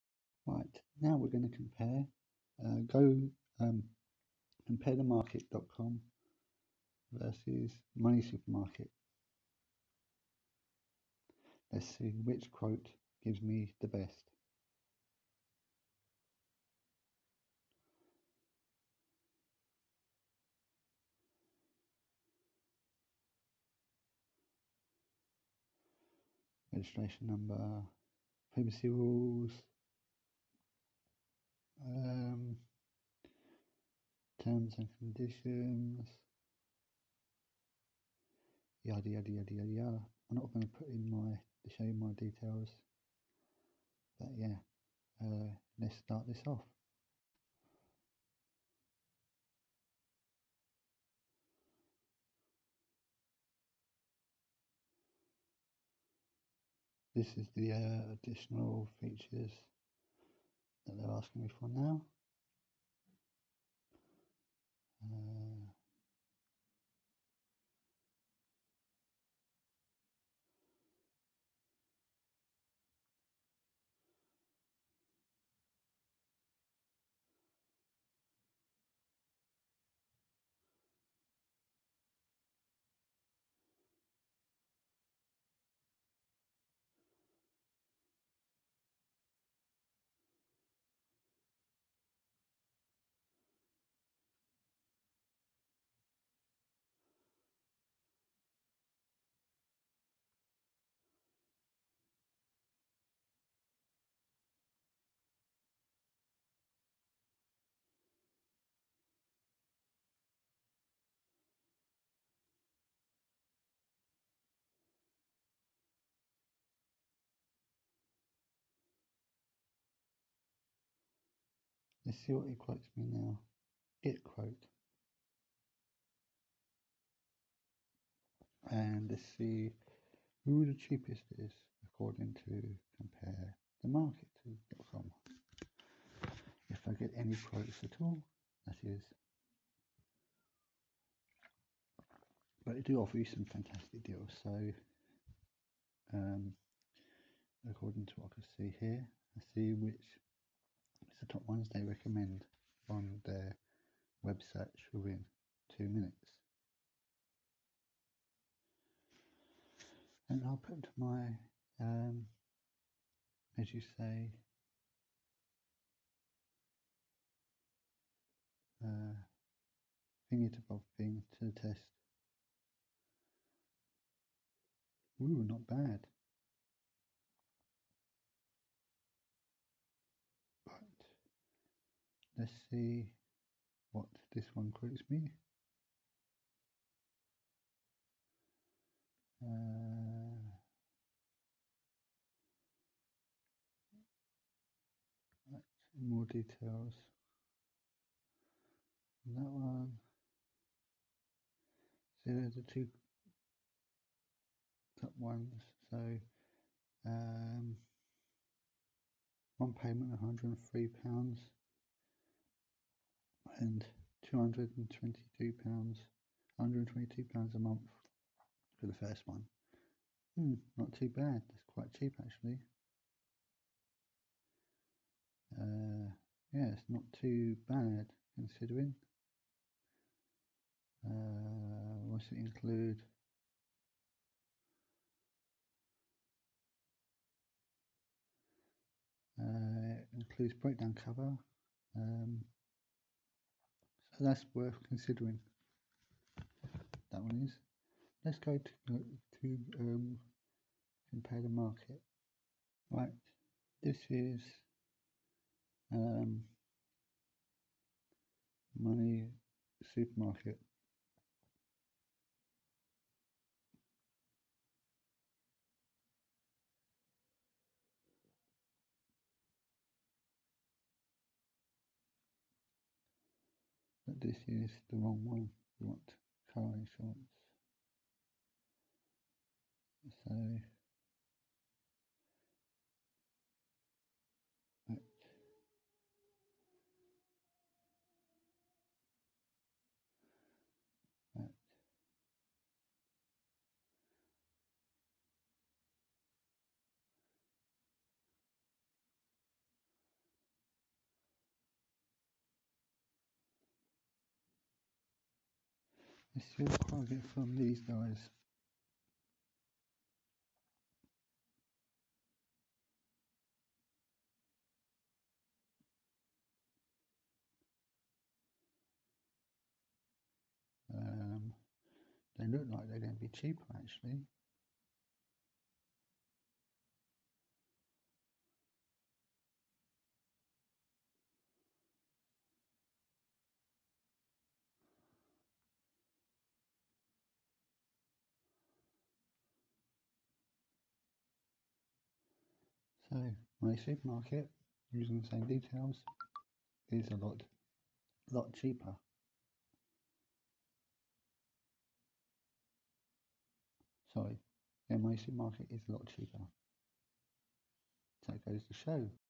Compare the market vs money supermarket insurance quotations
In this video I am comparing insurance quotations for motor vehicles in the United Kingdom to see who could actually save you money on the comparison sites only stay tuned to find out what my phone is are this video is not sponsored in any way or form